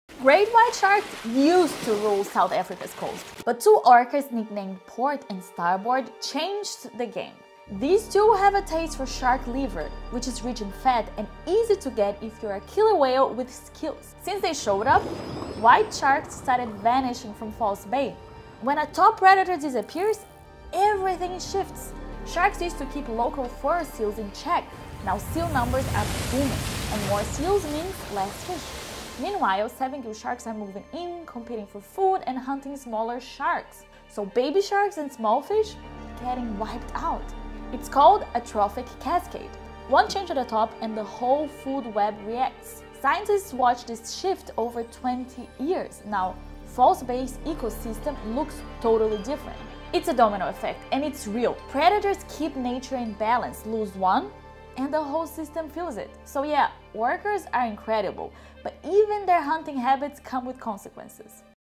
These Two Orcas claim White Shark territory for themselves! 🦈
Great white sharks once ruled South Africa’s coast… until two orcas, nicknamed Port and Starboard, arrived. These killer whales developed a taste for shark livers, driving white sharks away from False Bay and causing a massive ripple effect in the ecosystem.

With fewer sharks, Cape fur seals are booming, fish populations are shrinking, and smaller sharks are being hunted by sevengill sharks. This is called a trophic cascade—when one change at the top of the food web disrupts the entire balance of nature.

Over 20 years, False Bay’s ecosystem has transformed, showing just how powerful predators like orcas can be. Balance is key in nature, and even the ocean’s top hunters come with consequences.